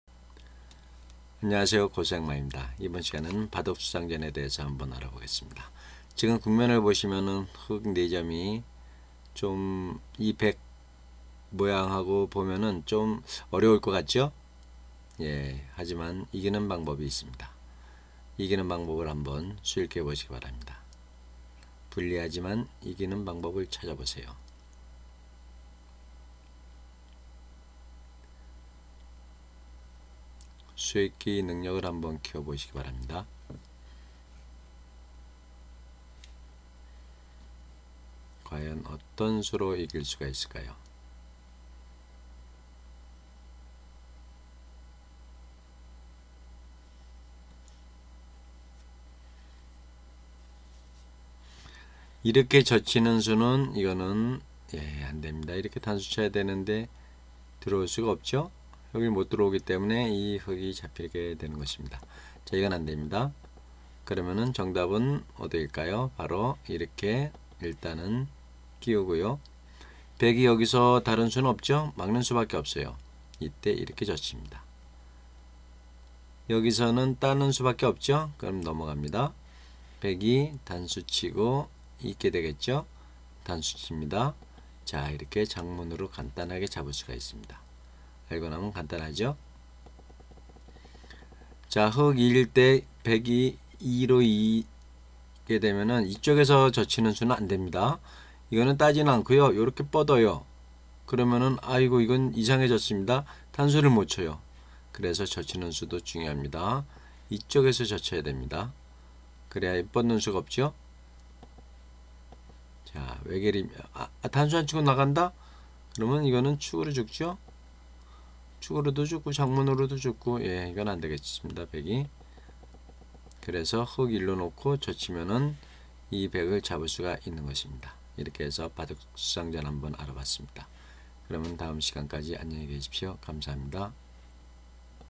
바둑수상전 - 불리한 형태에서 이기는 맥점 [수상전의맥]
수읽기가 필요한 대목입니다. 수상전에서 이기는
방법을 찾아보세요 [바둑수상전]